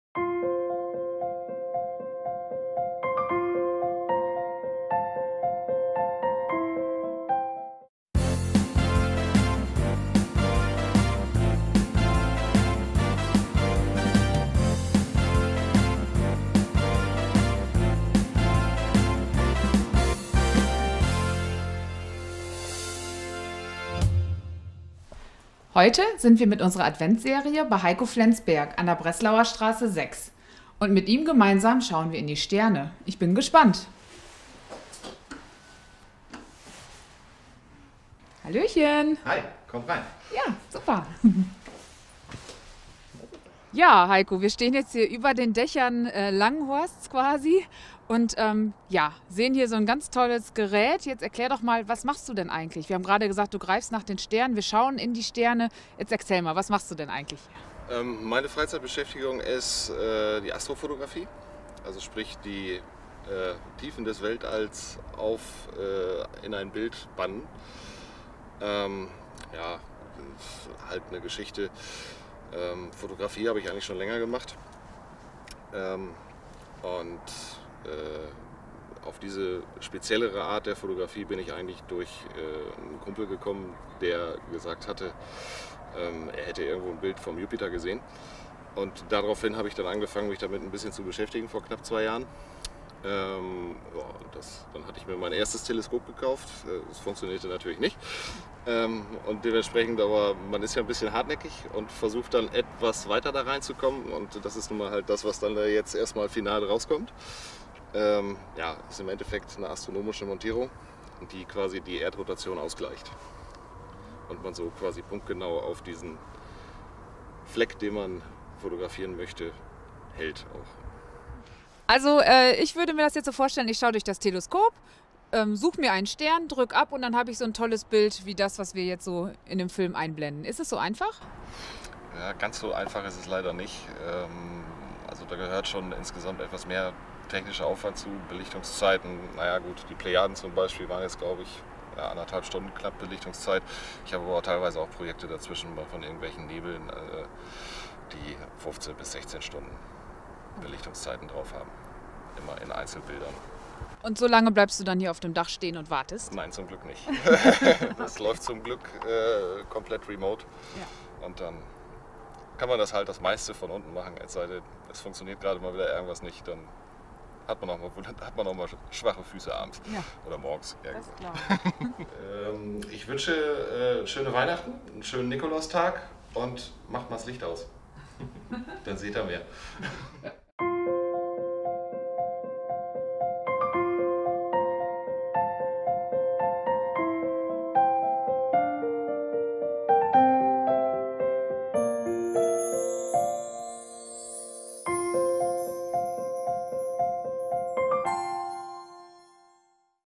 Adventsserie "Wo wohnst Du denn...?" Hausnummer 6
Gemeinsam mit Heiko Flensberg schauen wir in die Sterne und öffnen unser Türchen mit der Hausnummer 6 an der Breslauer Straße.